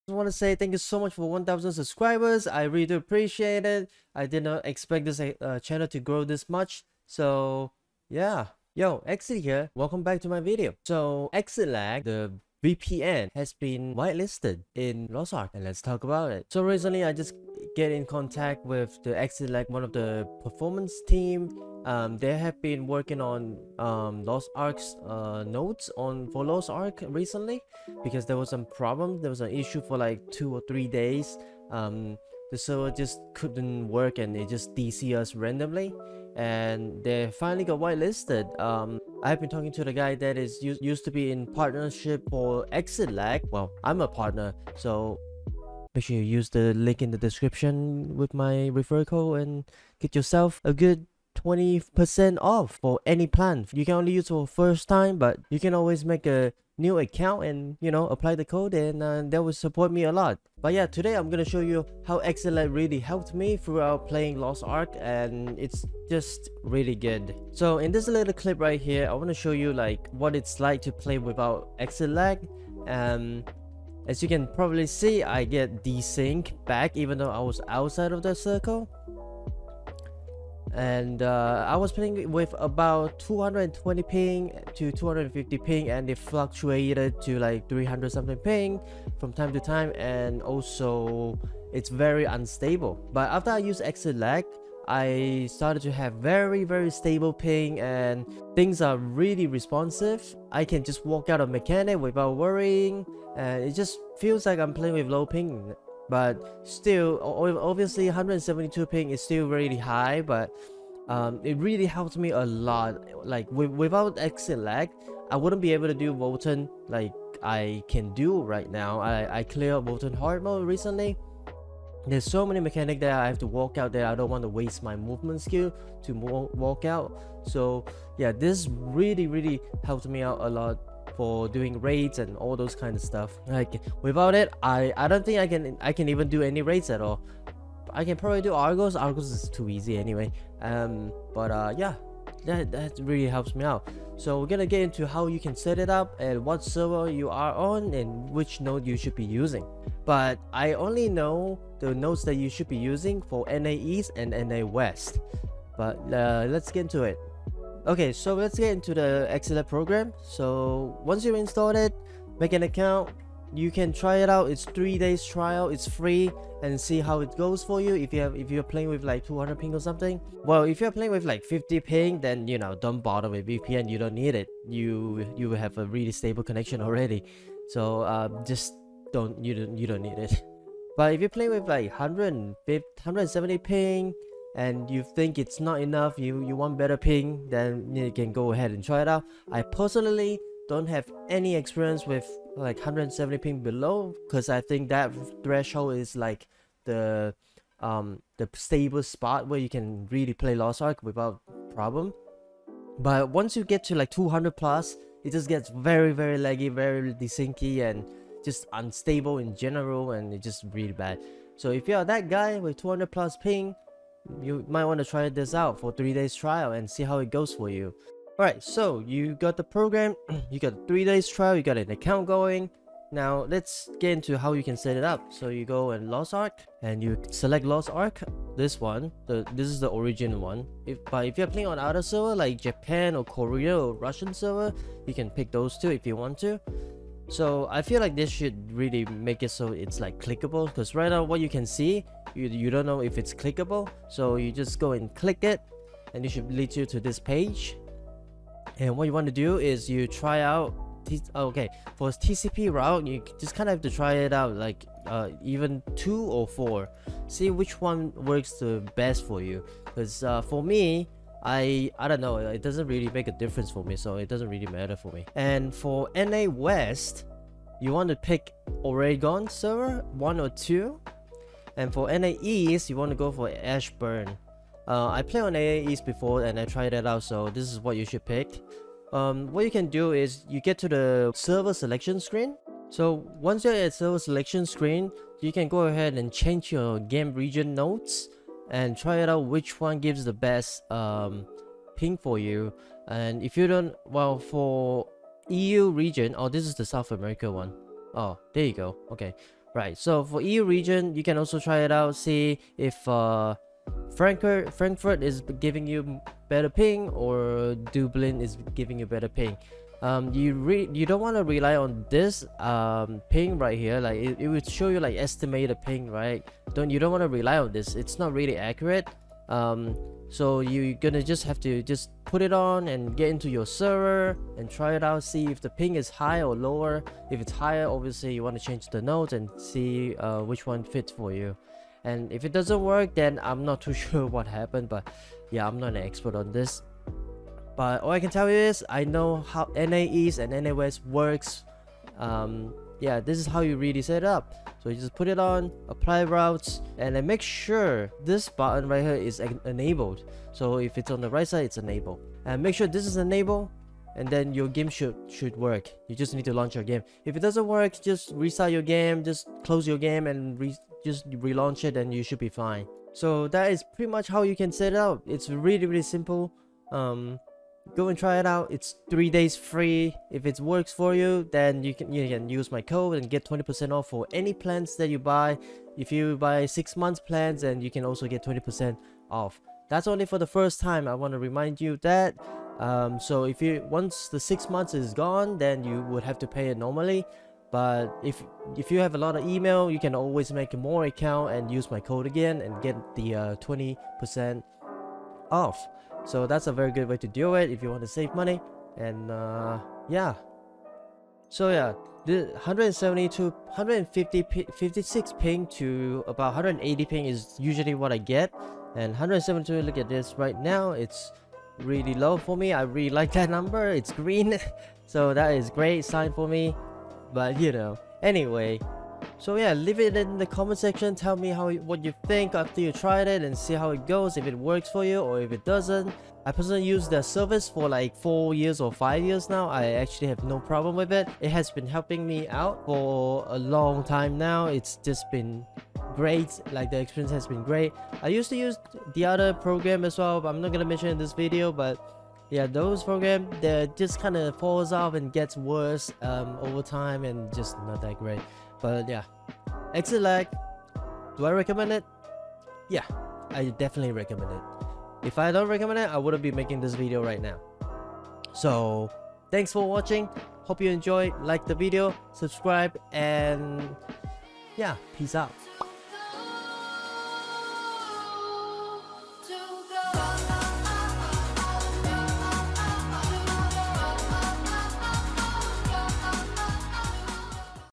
AMAZON WHITELISTED LOST ARK FOR ExitLag! HOW TO SET IT UP GET BETTER PING!
► [ ExitLag, reduce ping and get stable connection!]

Amazon just whitelisted ExitLag for NA Lost Ark! I believe players from OCE and other region is going to be very happy to hear this, since they really need it to play the game properly! i will be showing you how to set it up and you can try it out with the 3 DAYS TRIAL FOR FREE TOO!
Gear that i recommend using!

[Focusrite mic + interface package:]

[Hyper X Cloud 2 Headset:]

[240hz Acer Monitor:]

[Razer Keyboard:]